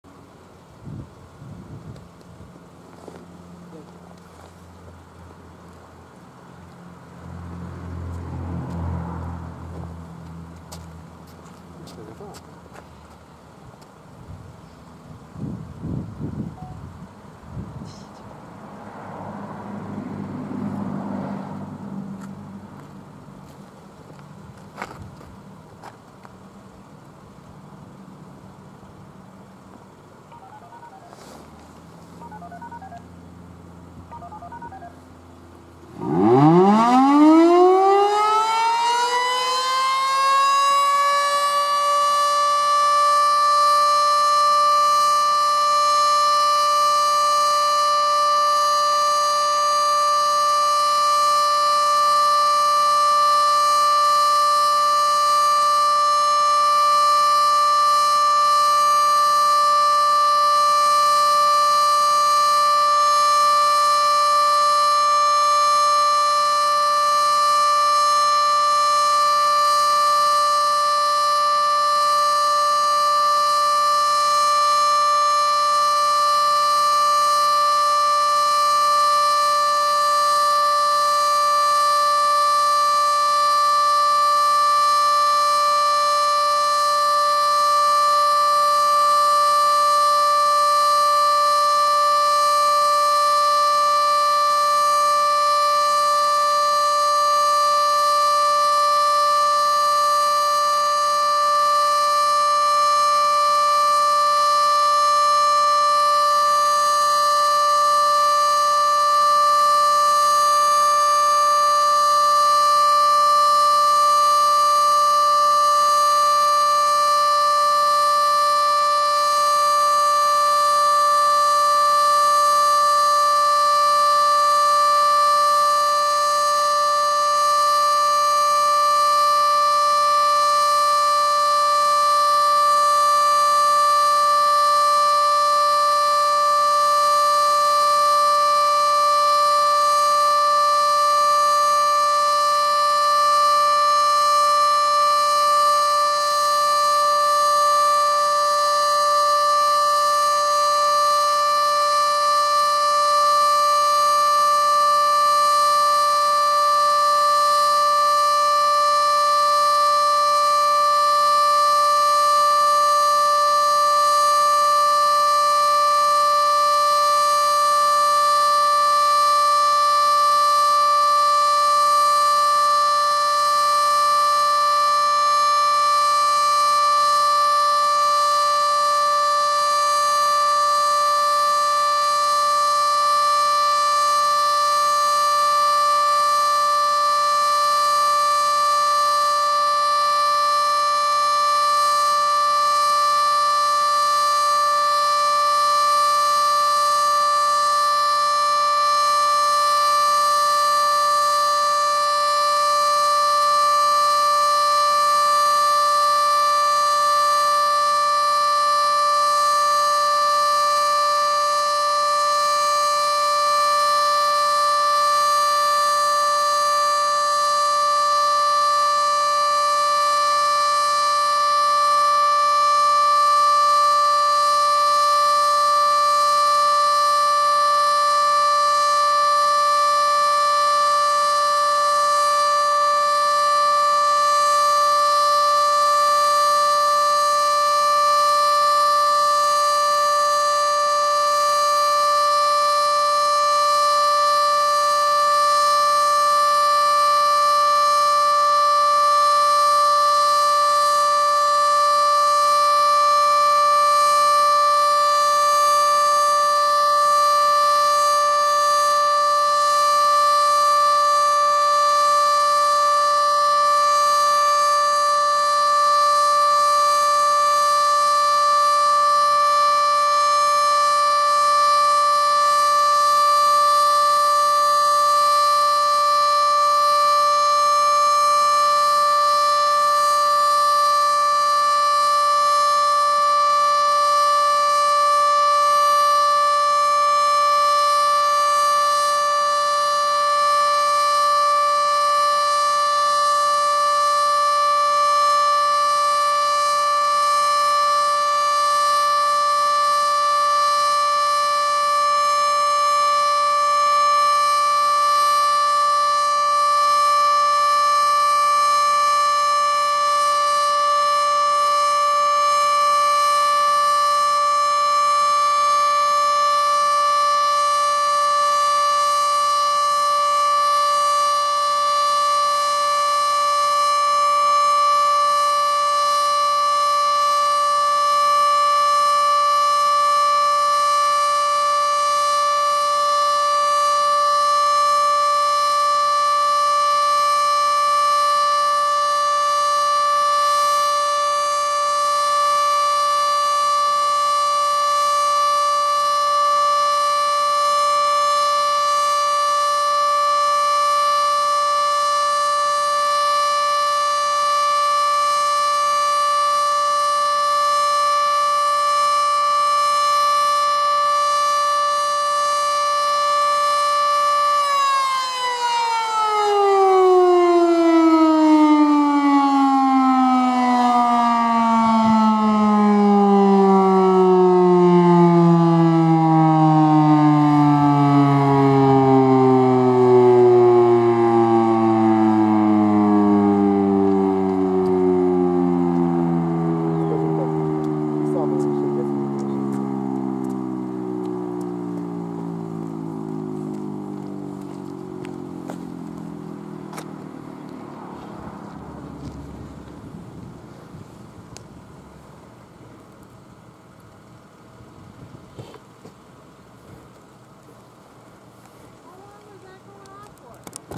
Dover, WI Federal Sign & Signal Model 7B (Racine Co. Siren Test) Full Alert
Decided to go see Racine county today, but I originally wanted to film Union Grove. When I arrived at the Roundback, I got a word it's switched off so I had to find a siren 10 minutes away which happened to be this Model 7B. It ignored all cancel tones and went for 5 minutes of alert. Elliot (STM1_Sirens) and Sam ( SirenJunkie2) also came out today but went to a 508. It was also James' (J.G.7 Sirens & More) birthday. Hope he had fun today! ;)
Anyway, Enjoy this rare Model 7B in full alert!

Other info
Testing date (Racine Co.): First Saturday of Every Month at 4pm
Equipment used: Panasonic HC V-550, Baofeng UV-5R Plus
Other Enthusiasts Present: James (J.G.7 Sirens & More), Chris (SirenDude1003), Milo (Midwest Mass Notification), Drew (Midwest Sirens) and Alex (WI Sirens And Elevators)
Location: Atop Dover City Hall, Dover WI